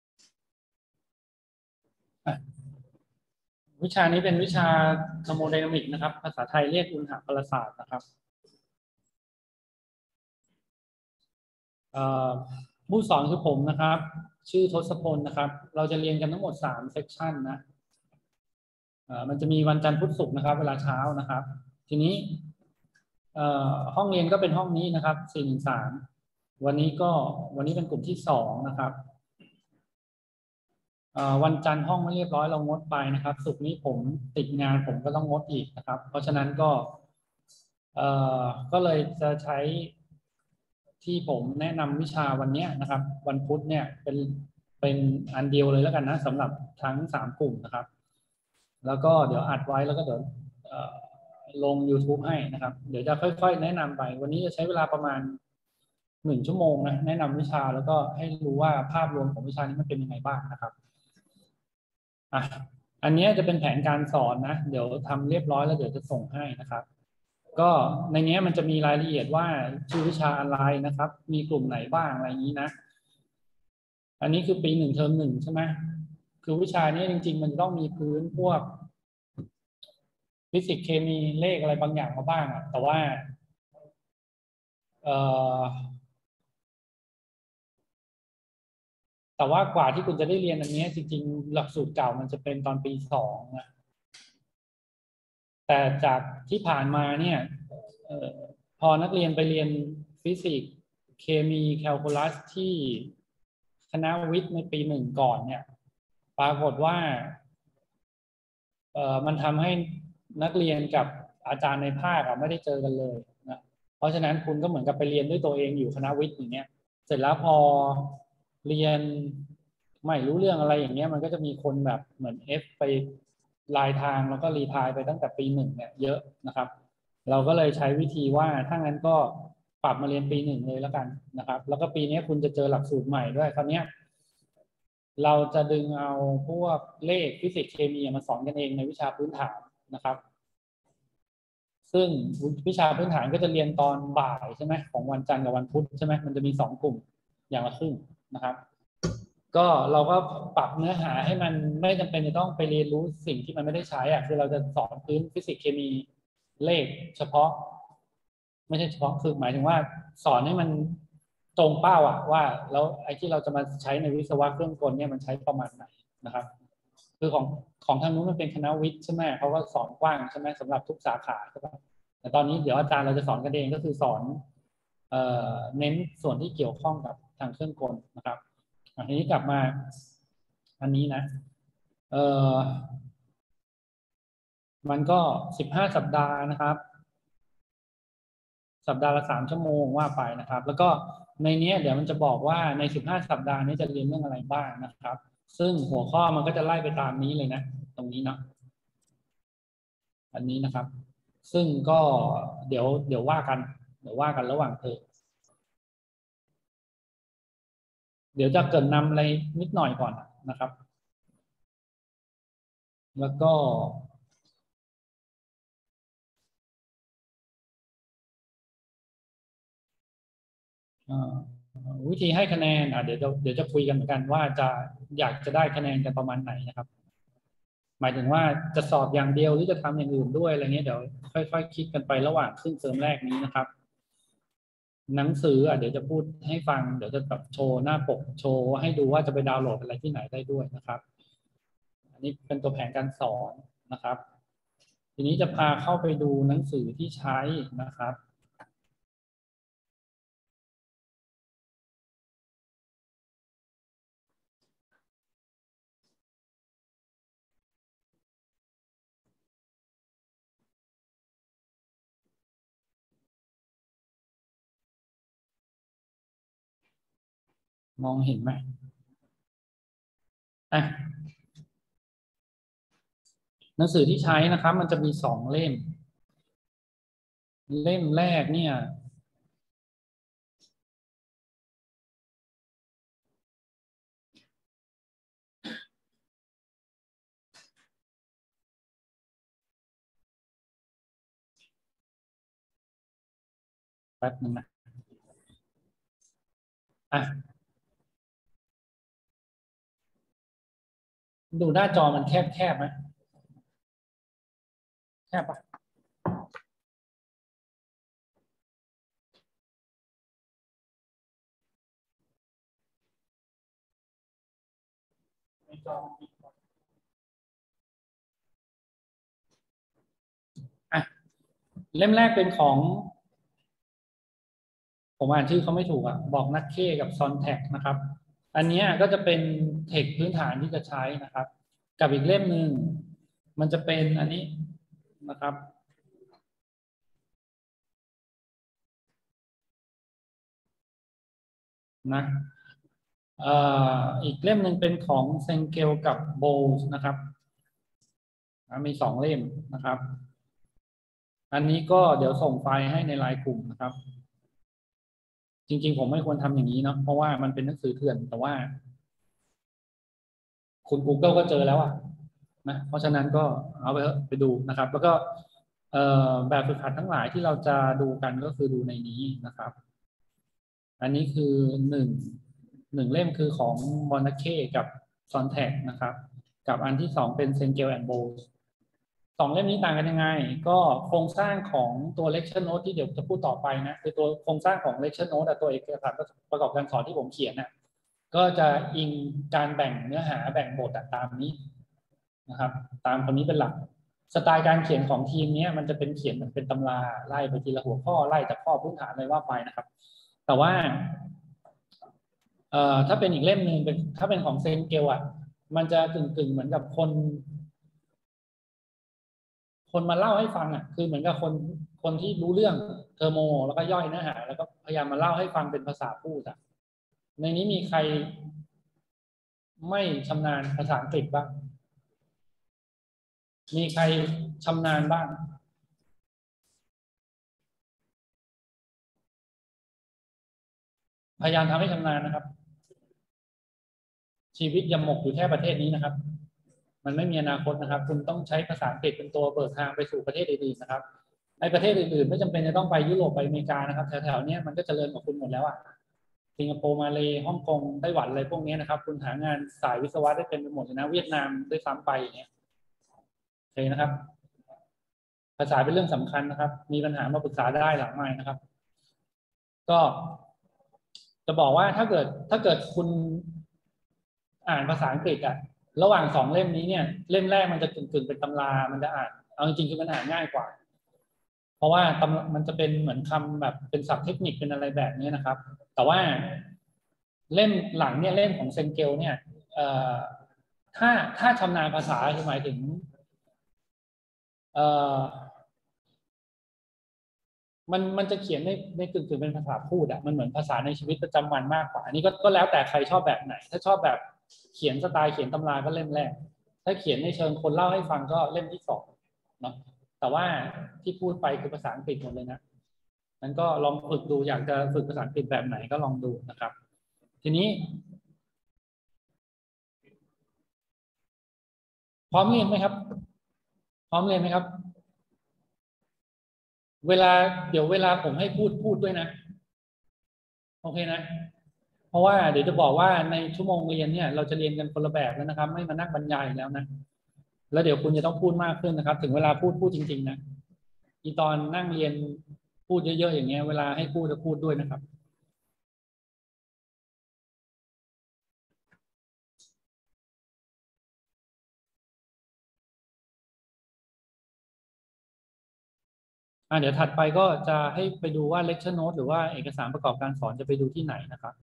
Thermo I 2565-1 S2 EP00-01 Intro
บันทึกการเรียนการสอนวิชา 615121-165 Mechanical Engineering Thermodynamics I
ภาคการศึกษาที่ 1 ปีการศึกษา 2565
กลุ่มที่ 2

บันทึกเมื่อ: พุธ 6 ก.ค. 2565

ผศ.ดร. ทสพล เขตเจนการ